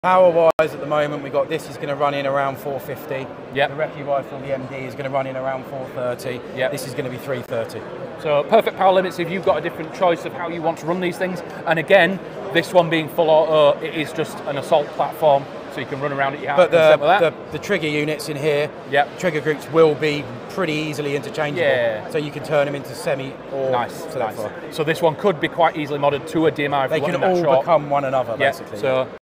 Powerful and Versatile: Modifying the Ultimate Assault Platform
Unlock the true potential of this incredible assault platform and learn how to easily customize it for different functions. Whether you prefer full auto or semi-auto, this video will show you how to upgrade and optimize your weapon.